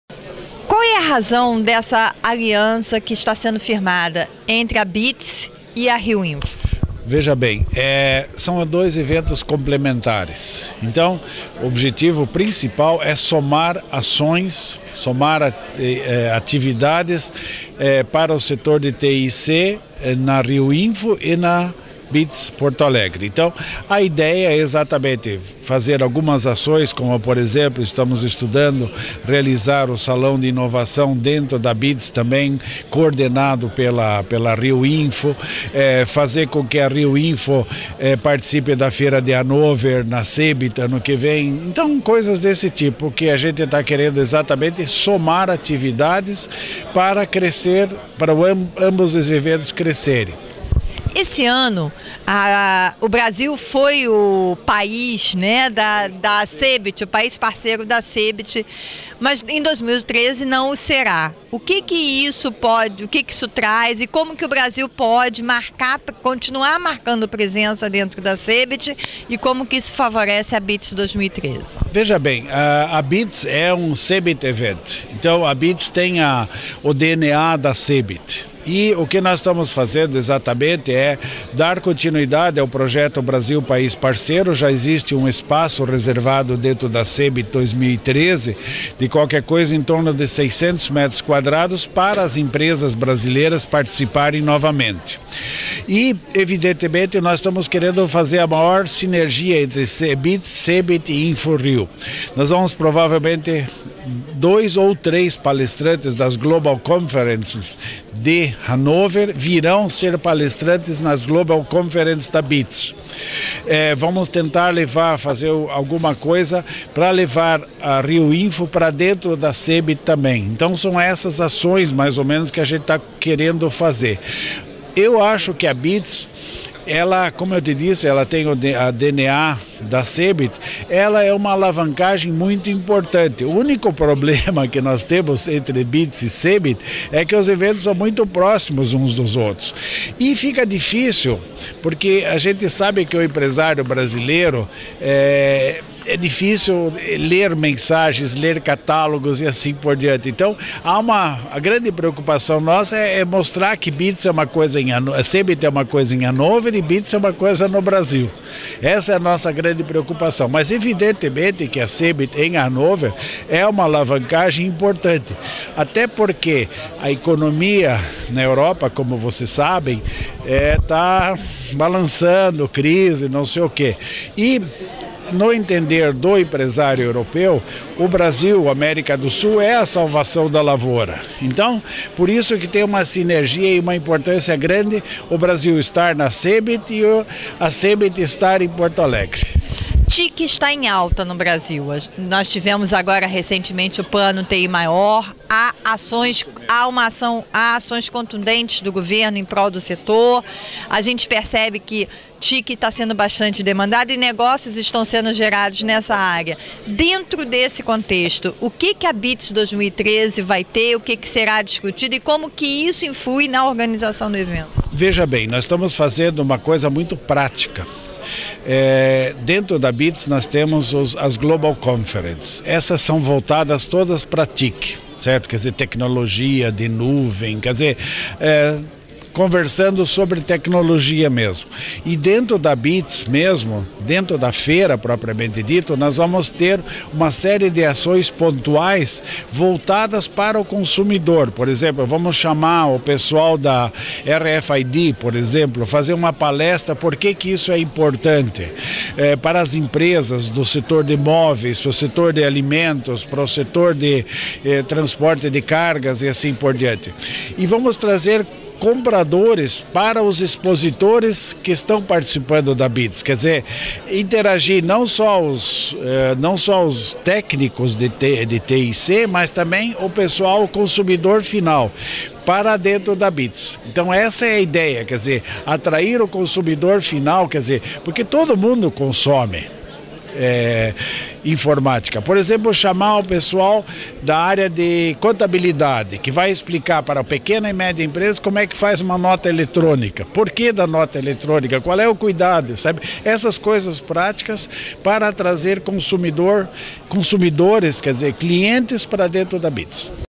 BITS 2013 e Rio Info somam ações pelo setor de TIC
Evento, que será realizado de 14 a 16 de maio, em Porto Alegre, fechou parceria com o Rio Info para a troca de experiências. Salão da Inovação será replicado no evento gaúcho.

A informação foi passada pelo diretor da Hannover Fairs Sulamérica, Constantino Baümle, em entrevista à CDTV, do Convergência Digital. Segundo o executivo, a BITS e o Rio Info são complementares e podem somar para o setor de TICs.